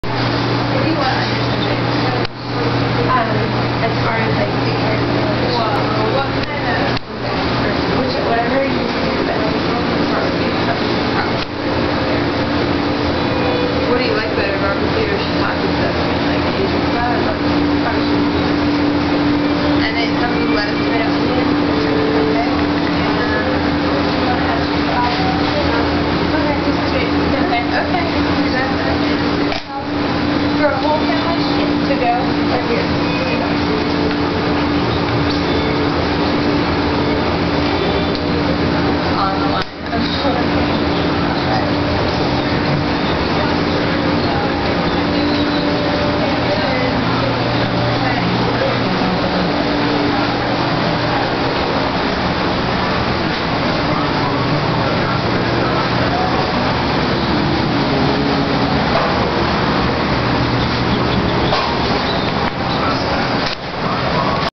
Health Foods in Eugene Oregon
Getting some organic Heath Foods in Eugene.